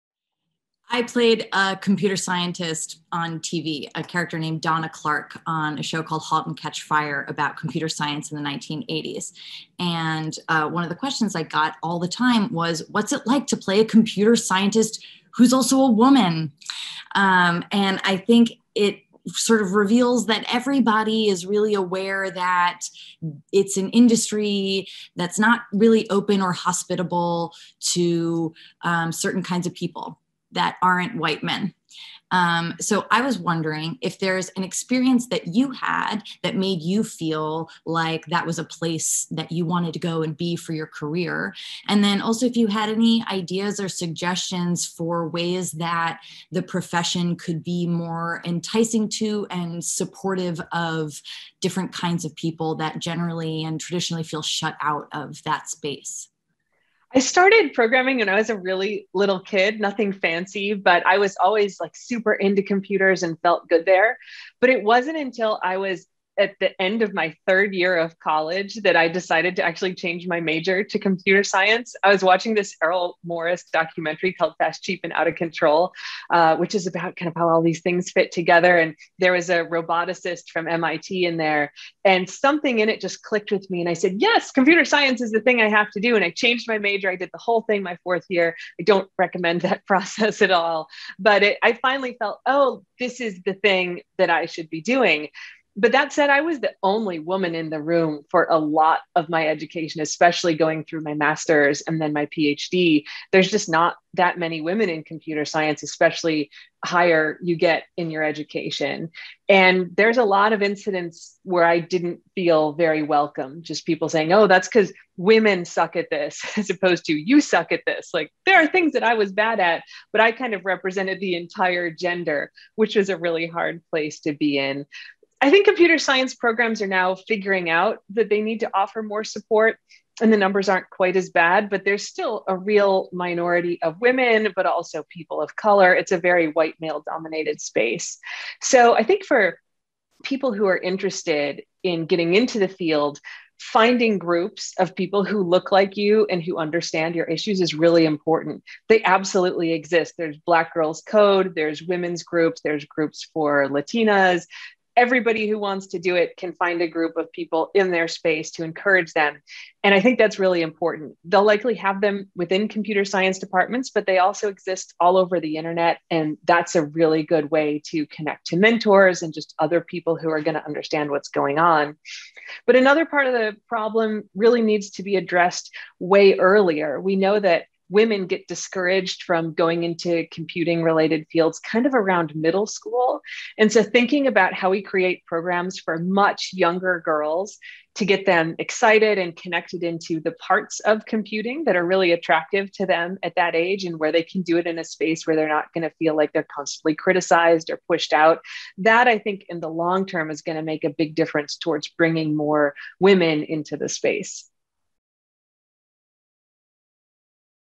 ASK A SCIENTIST...about being a woman in computer science
Actor Kerry Bishé, known for her lead role as Donna Clark, a computer scientist, in the AMC television series Halt and Catch Fire talks with Dr. Jen Golbeck, computer scientist. Jen currently is a Professor at the College of Information Studies, an affiliate professor in the Computer Science Department, and an affiliate professor in the Journalism Department, all at the University of Maryland, College Park.